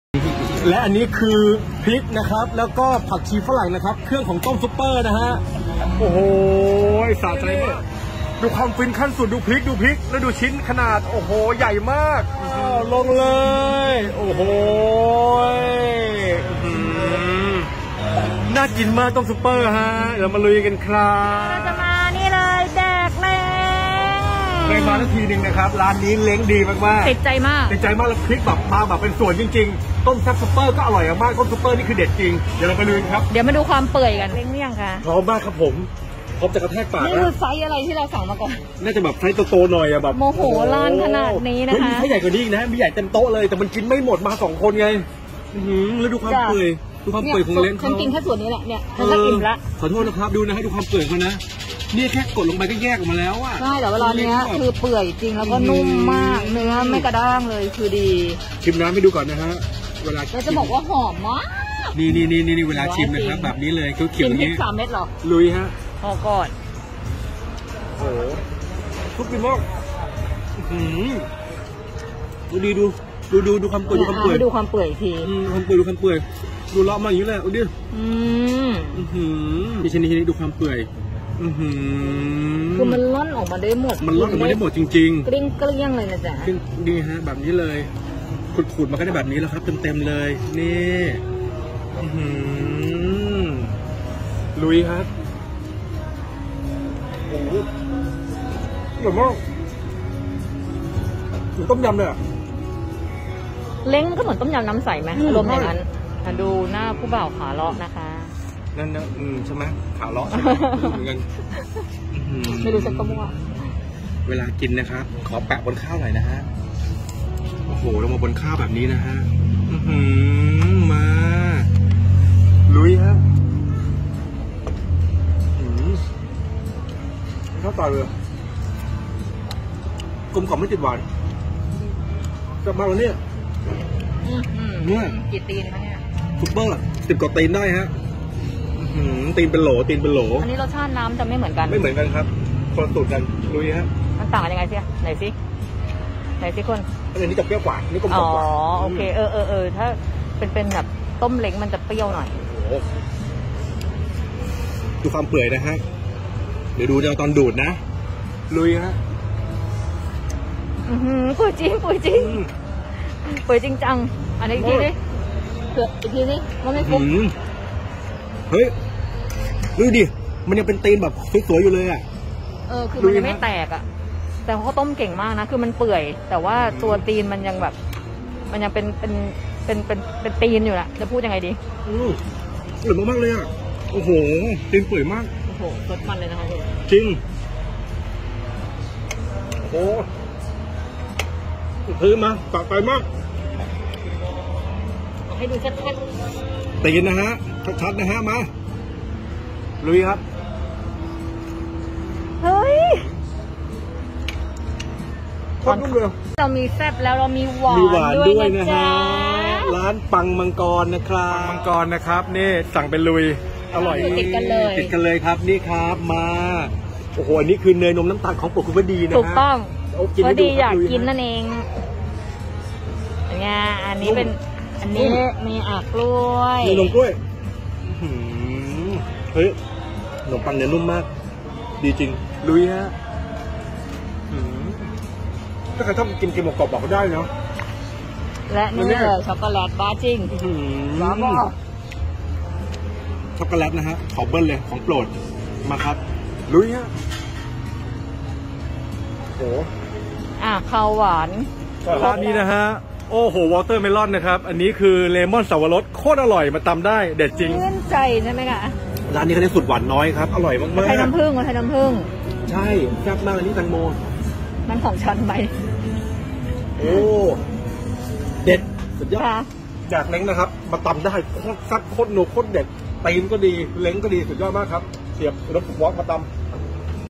ต้มแซ่บเล้งรสจัดจ้าน เปื่อยๆ นุ่มๆ แซ่บๆ นัวๆ ต้องโดน!!!
"มาร้านนี้ก็ต้องต้มแซ่บเล้ง กับต้มซุปเปอร์ดิค๊าบบบ
เปื่อยๆ นุ่มๆ แซ่บๆ นัวๆ
ร้าน : แดกเล้ง
พิกัด : อุดมสุขวอร์ก
เมนู : ต้มแซ่บเล้ง ต้มซุปเปอร์
ราคา : 100 บาทขึ้นไป
เวลา : 17:00-21:00 น.

ติดตาม อปป้า ผู้สรรหา อาหารเด็ด มาจุดประกายความหิวยามค่ำคืนได้ที่